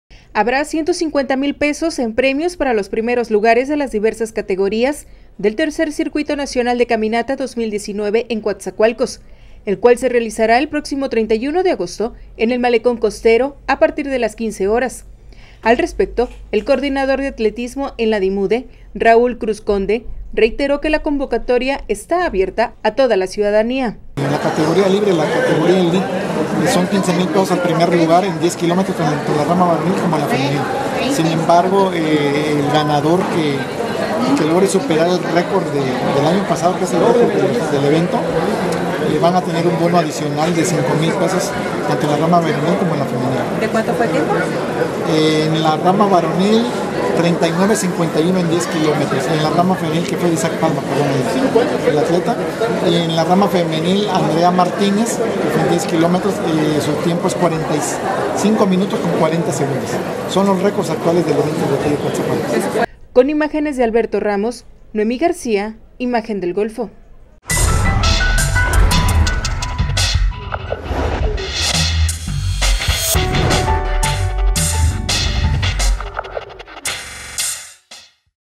Presentan Tercer Circuito de Caminata en Coatzacoalcos
Habrá 150 mil pesos en premios para los primeros lugares de las diversas categorías del Tercer Circuito Nacional de Caminata 2019 en Coatzacoalcos, el cual se realizará el próximo 31 de agosto en el Malecón Costero, a partir de las 17:00 horas.